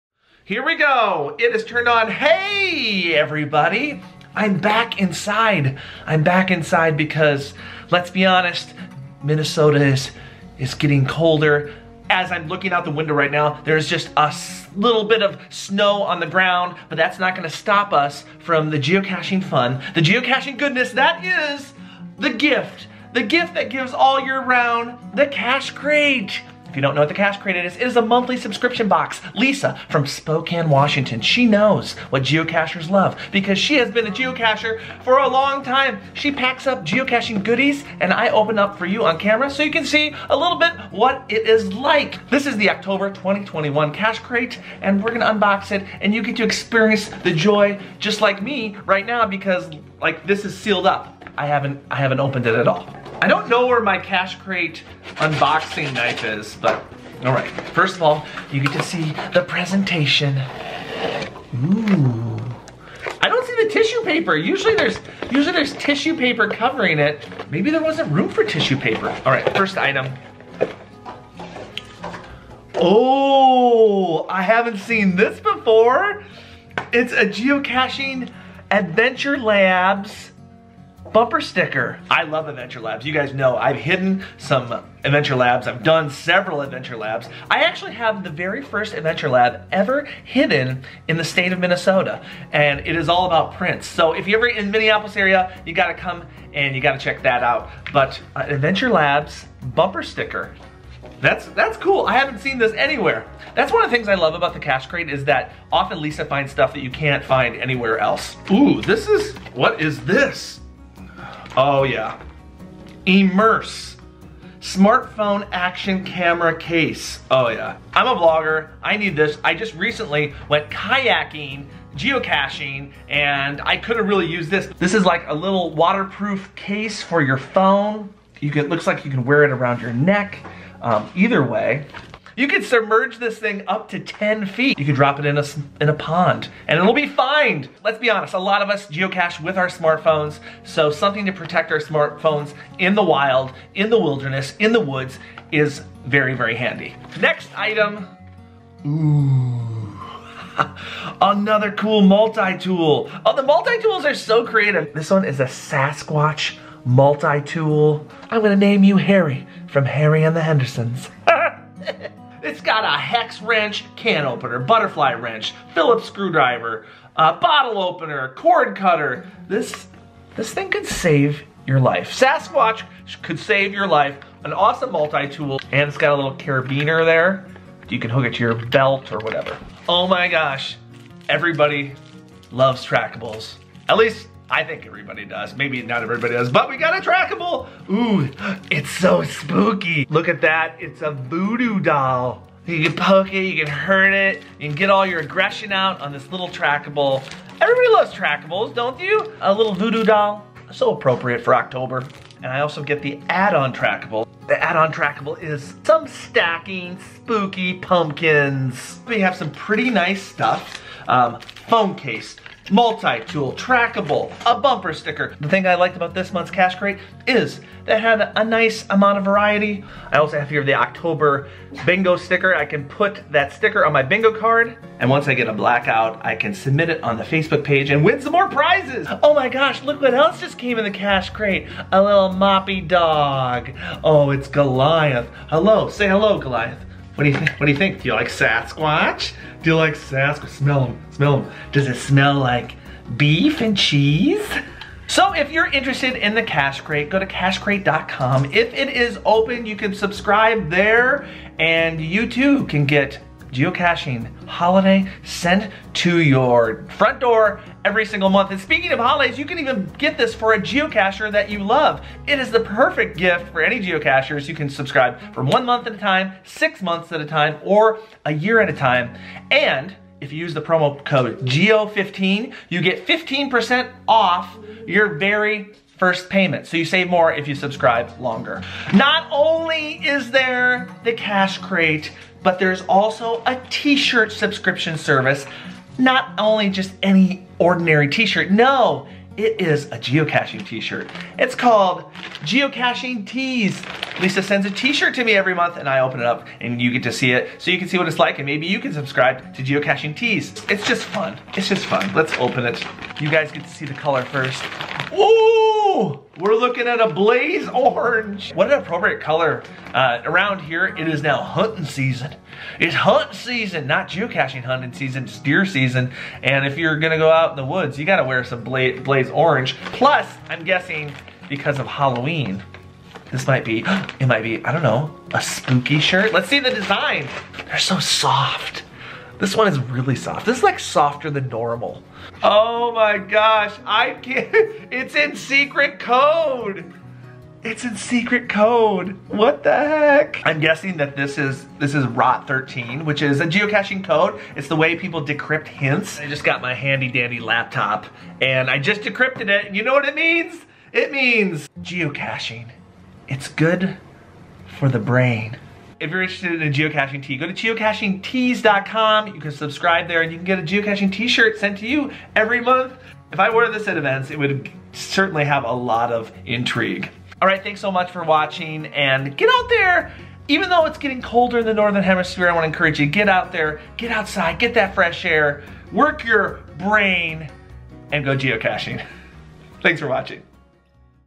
A GEOCACHING SURPRISE EVERY MONTH! (Oct 21' Cache Crate Unboxing)
I love that I get a fun geocaching surprise every month thanks to the Cache Crate! Watch as I open the October 2021 Cache Crate and Geocaching Tees Subscription Service.

Thank you Cache Advance for partnering with us in this video.